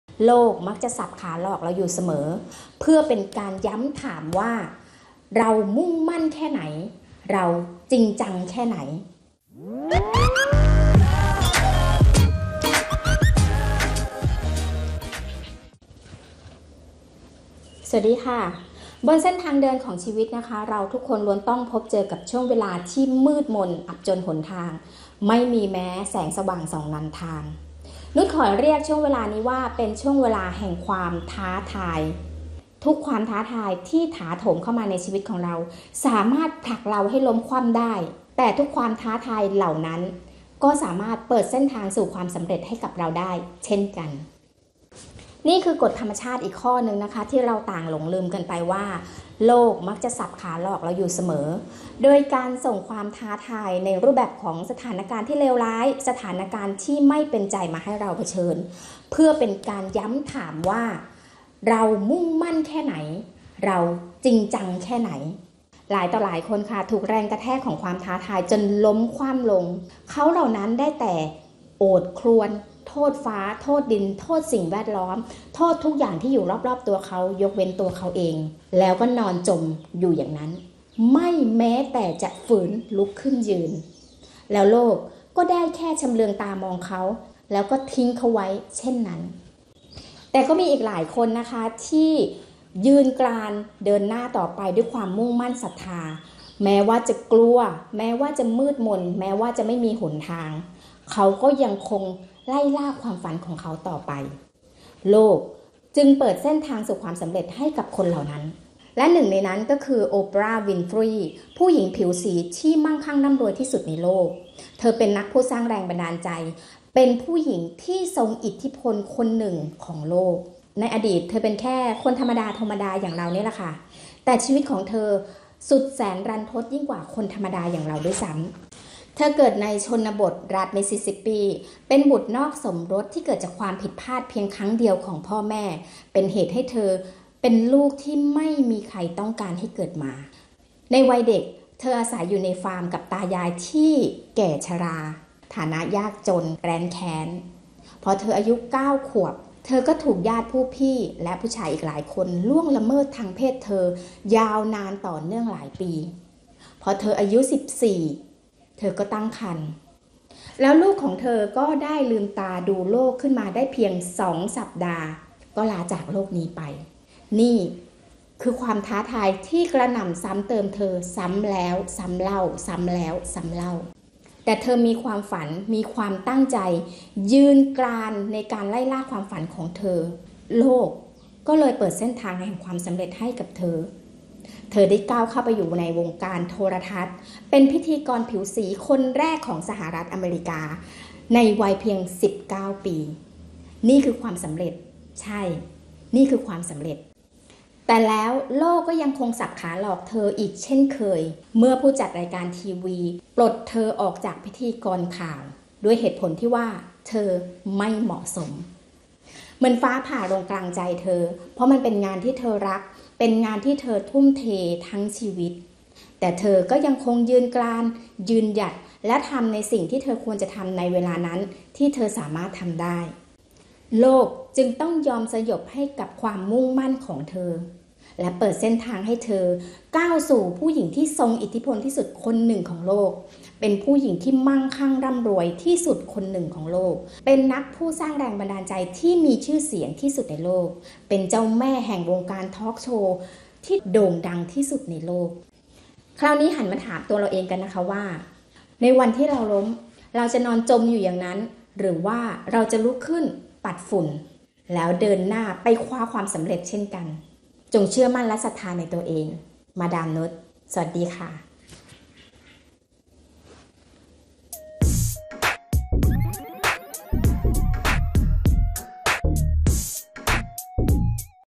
เราถูกโลกทำร้ายหรือโลกกำลังให้อะไรเรา/โลกมักจะสับขาหลอกเราอยู่เสมอ
เราถูกโลกทำร้ายหรือโลกกำลังให้อะไรเรา โลกมักจะสับขาหลอกเราอยู่เสมอ โดยการส่งสถานการณ์เลวร้ายมาให้เราเผชิญ  เคล็ดลับก้าวผ่านชีวิตพังๆ สูงความสำเร็จ ตามแบบฉบับของโอปราห์ วินฟรีย์ จากเด็กที่มีชีวิตรันทด สู่ผู้หญิงที่ทรงอิทธิพลที่สุดในโลก